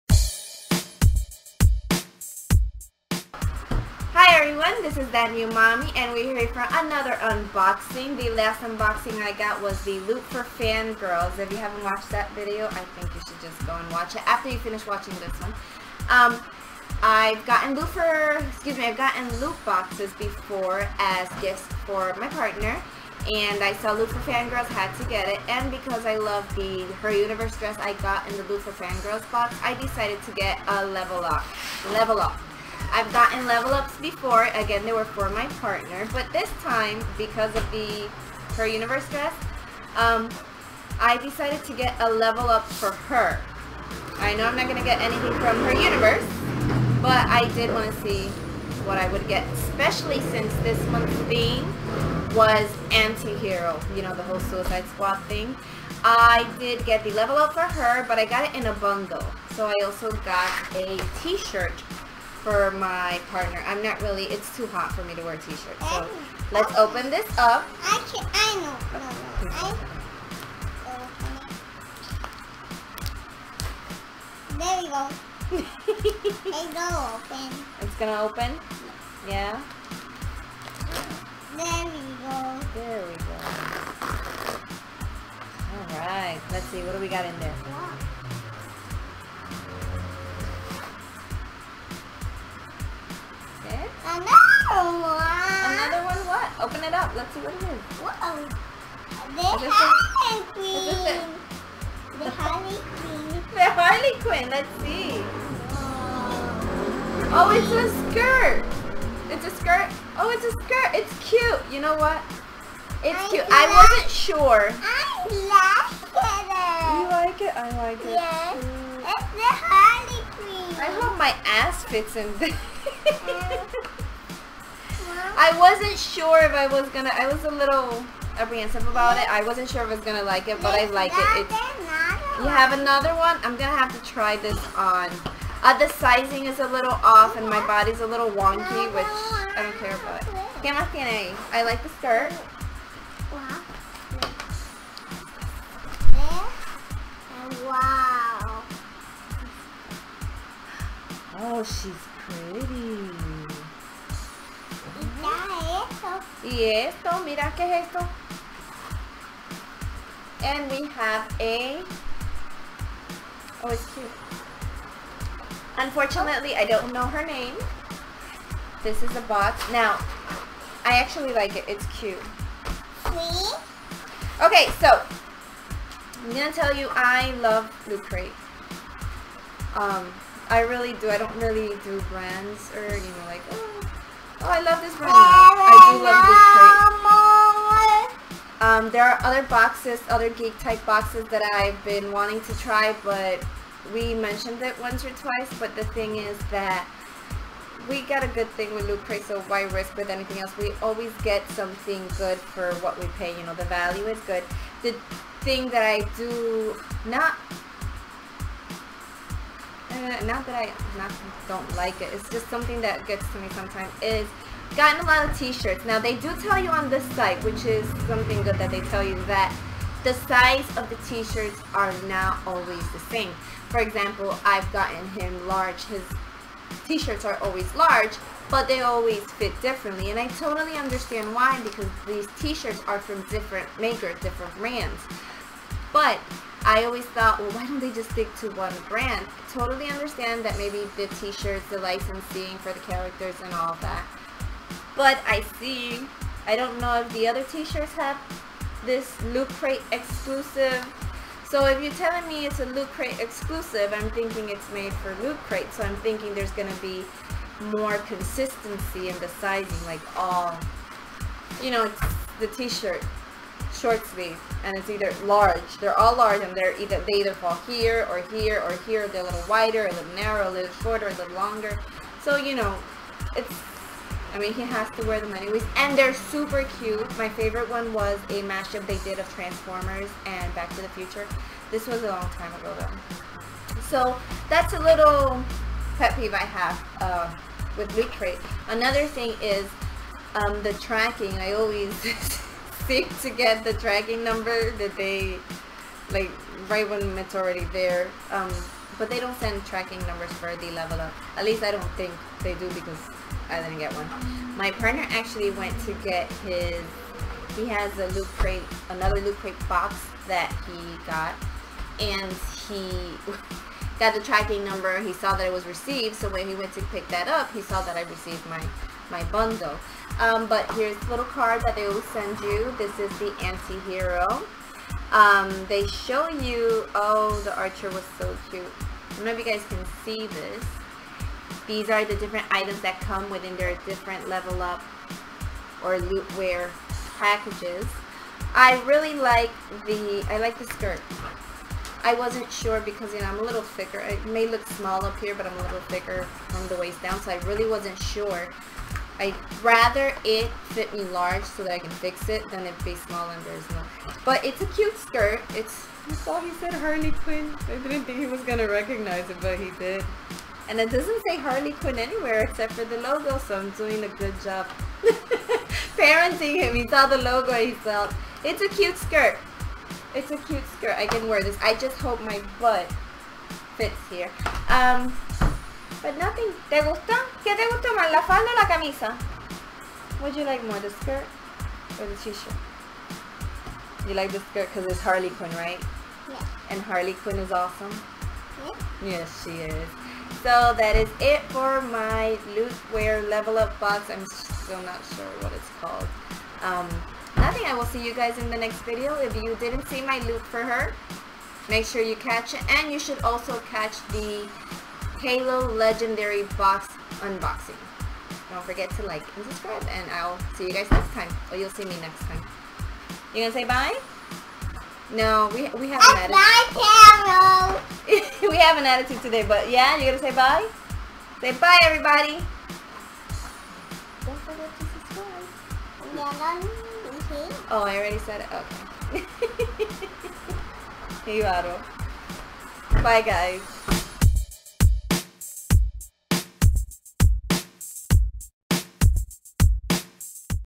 August Unboxing -- LvlUp Loot for Her & Loot Tees
First things first: Her name is Sylvanas Windrunner aka the Dark Lady aka Queen of the Forsaken. (＾ᗨ＾✿) My first Lootwear (previously LvlUp) Loot for Her crate!  I bought it in a bundle with Loot Tees.

When I buy from Amazon, I use Amazon Prime.